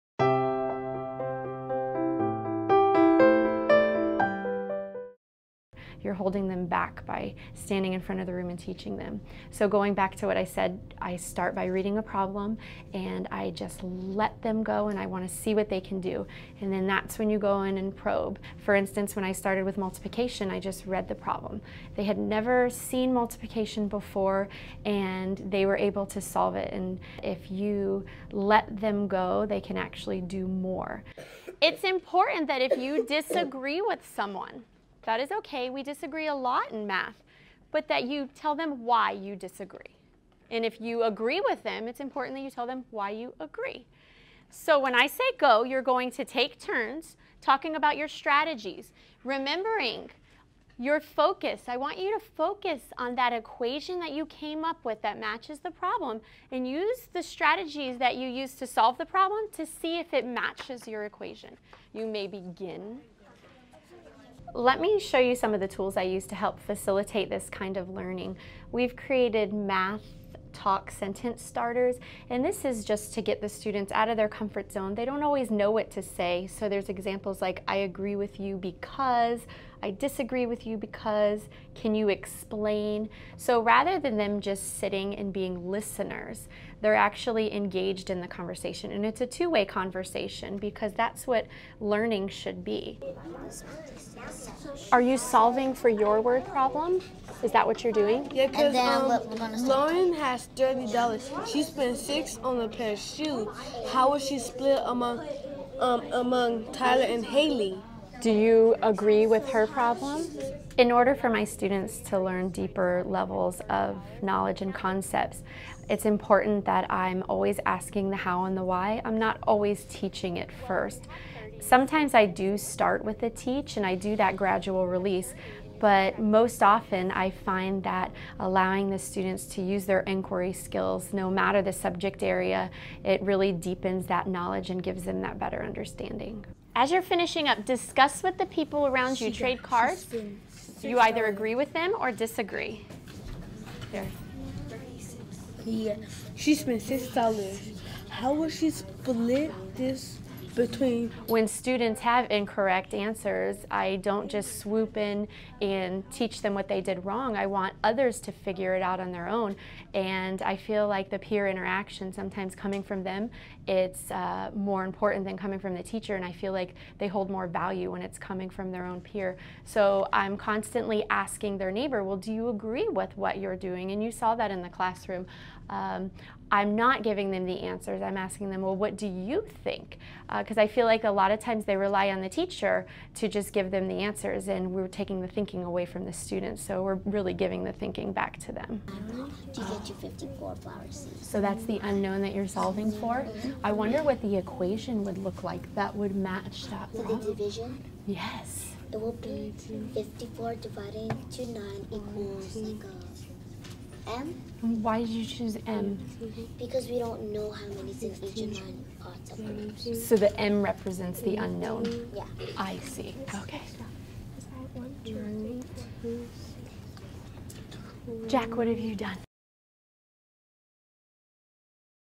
Accountable Talk
Elementary teacher modeling accountable talk strategies in a math class.